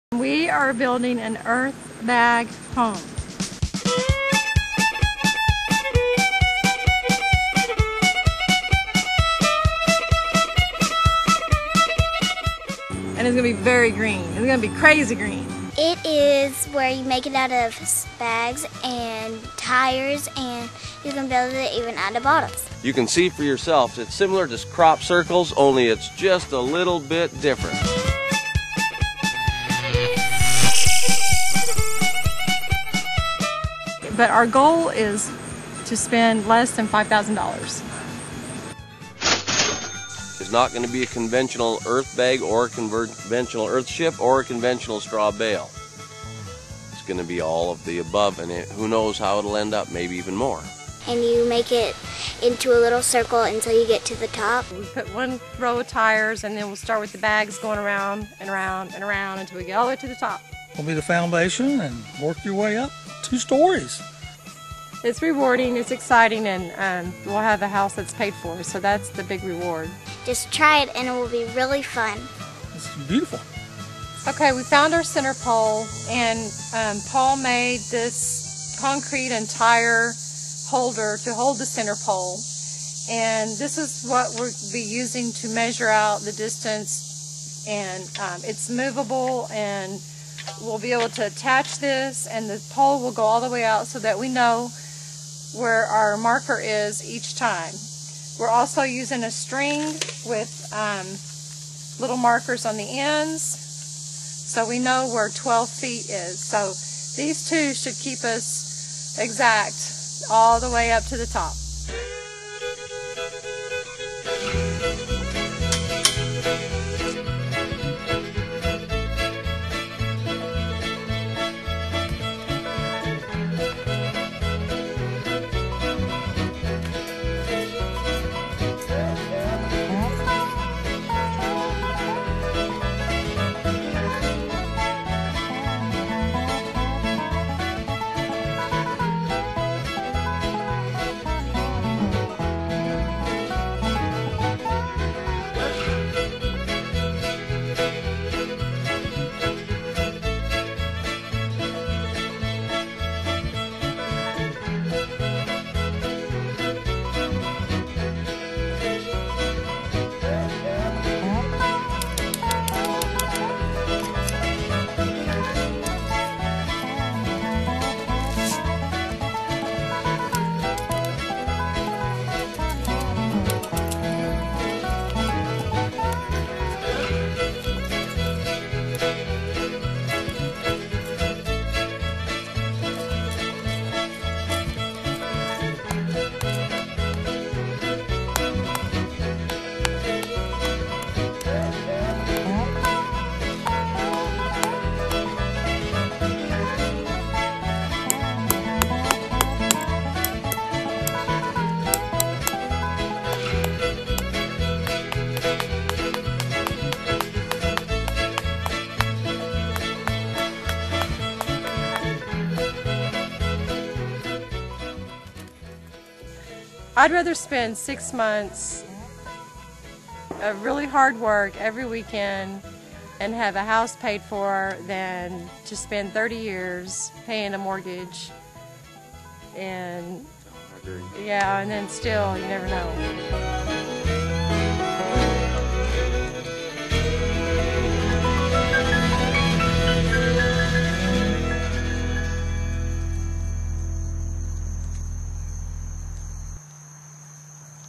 Earthbag Home in Arkansas - Packing Tires - 2 MAJORS
An Earthbag Home is made of old tires, sand bags, dirt, bottles, and other recycled materials. Paul and Lisa are building their first earthbag home in Garfield. In this video they make a measuring device out of fencing material to keep the first row of tires level. They are using SP2 or "dirty gravel"  to fill and pack the tires. The next video will show them filling sandbags and creating window forms and door posts.